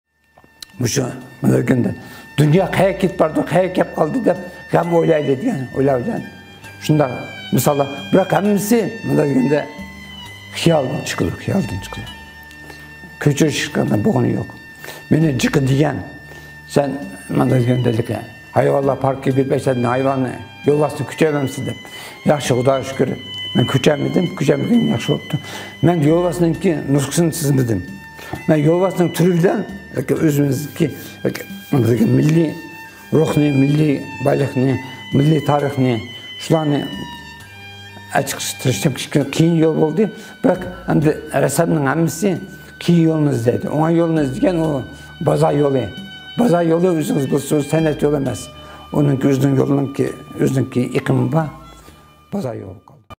drawing a tiger and the Uyghur spirit
Abdulkerim Isa is one of the most famous Uyghur painters, who lives in Almaty, Kazakhstan. He was born in Ghulja, East Turkistan. He is specialized in painting of tiger because he is engaged in it about 20 years. He has drawn about 1700 works so far and some of his works received golden award and certificate from France. His works mainly depict the national spirit, history and spiritual wealth of Uyghur people. As for his career, he said: It was a difficult job. However, a real painter selects the difficult road. The easy road is the road to the market. The road to

market, you know, is not the road of the art. It has its own road. It isn’t the road to the market.